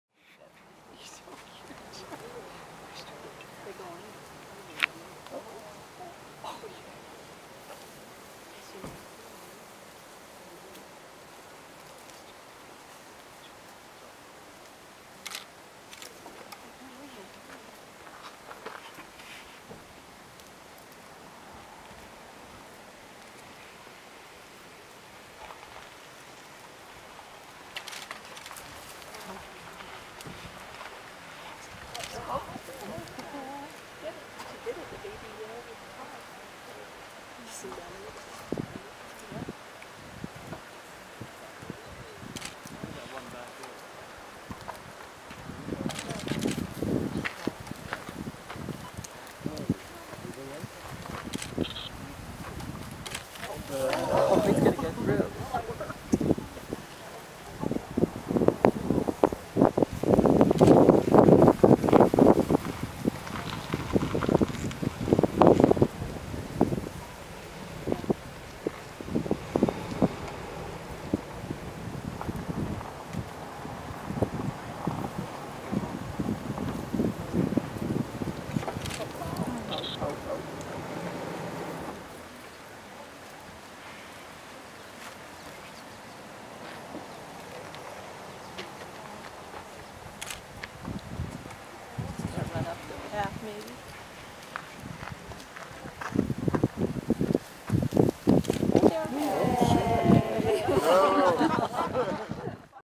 Triumph of Little Bighorn
A small flock of Bighorn Sheep negotiate a fence, apart from one youngster.  To the delight of a crowd of about 30 he finally succeeds.

Videod on 21 September 2008 on an Ornitholidays trip by my wife Chris near Tower in the Yellowstone National Park.